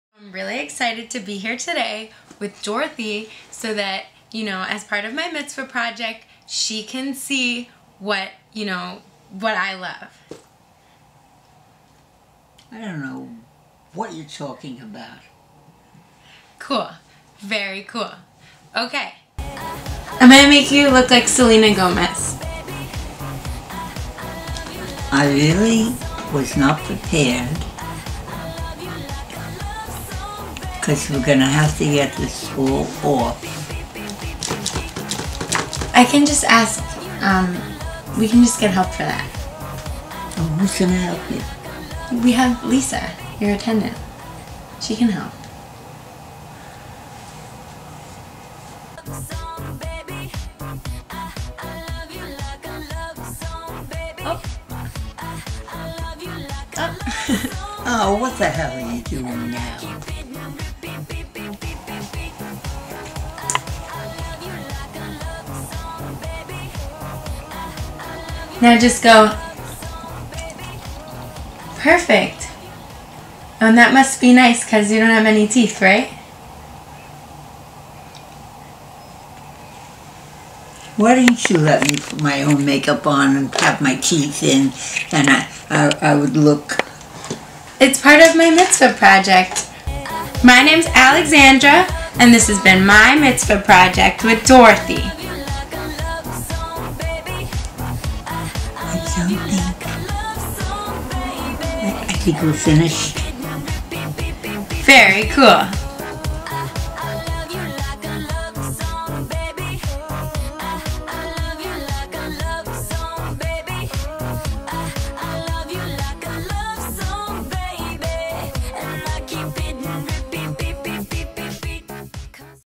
ALEXANDRA'S MITZVAH!
Alexandra Levin, a recent Bat-Mitzvah gives her friend Dorothy a make over!

She loves Selena Gomez and being cool!